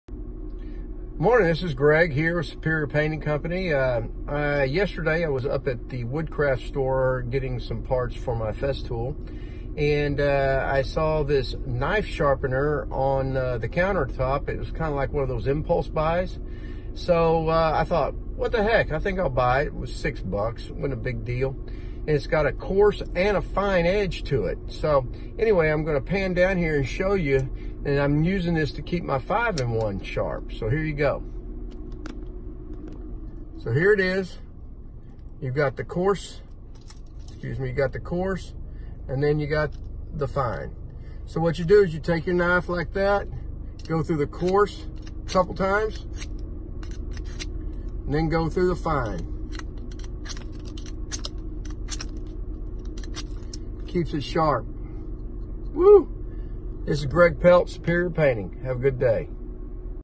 Superior Painting Company Tip: How to sharpen your 5 in 1 Painting Tool
Superior Painting Compnay Tip:  How to sharpen your 5 in 1 painting tool.
Residential and Commercial Painters in East Dallas.